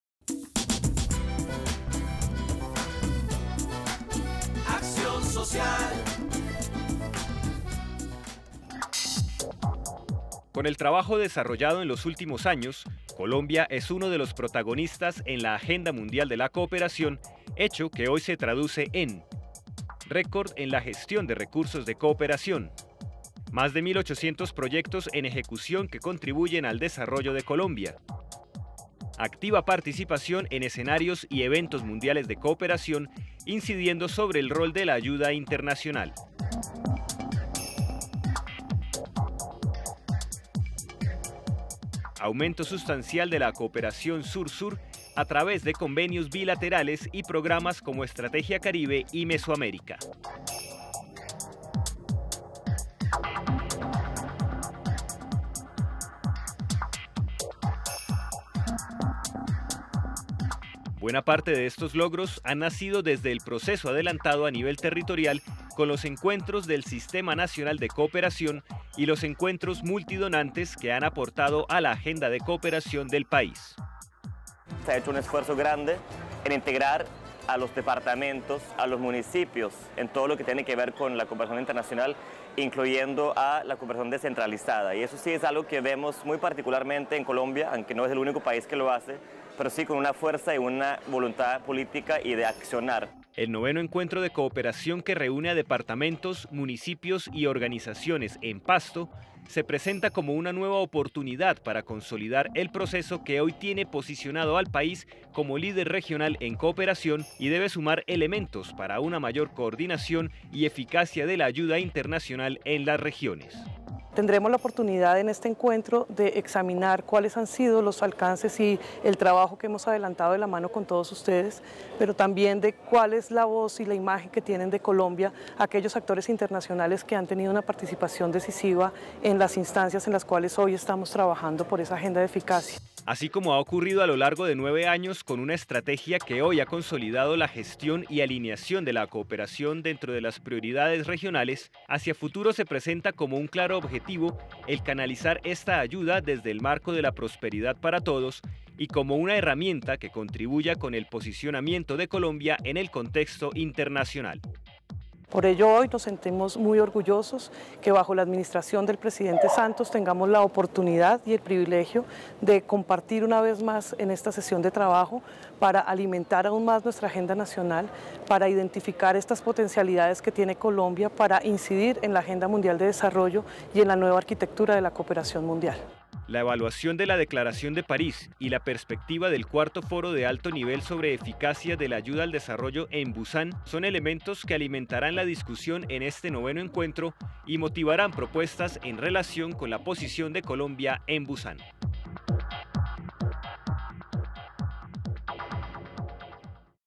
IX Encuentro del Sistema Nacional de Cooperación Internacional
Países, agencias internacionales, gobernaciones y alcaldías debatirán en torno a la eficacia de la cooperación. La Emergencia Invernal desde la perspectiva de la cooperación internacional otro de los temas que se presentarán. En Nariño hay presencia de 23 cooperantes internacionales.